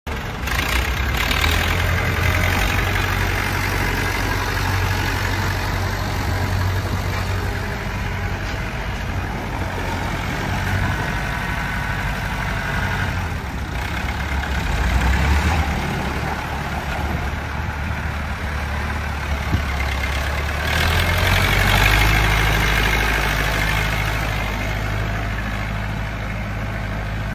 MASSEY FERGUSON 175 For Sale
2WD, 3PT HITCH, 540 PTO, DUAL REMOTES, DIESEL ENGINE, POWER STEERING, REAR WHEEL WEIGHTS, STABILIZER BARS, SHOWING 4540 HRS, S#SDW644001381, TAG#27606